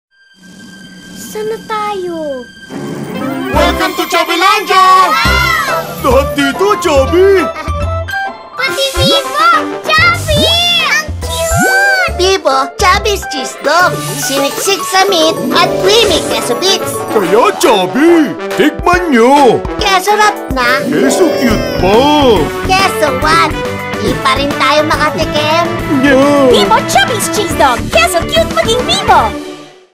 Bibbo Chubbies Hotdog "Wonka" TV Commercial 30s (2007)
FOR PURPOSES OF PROMOTING THE CREATOR'S WORK ONLY.
See attribution below.

ATTRIBUTION:

CLIENT
CDO Foodsphere Inc.

AGENCY
Y&R Philippines
Bennet Dychangco (Creative Director)
Mike Fallarme (Art Director)

★ Creator reserves the right to use the creation in any manner, at any time and in any part of the world for the purpose of advertising or otherwise promoting the work.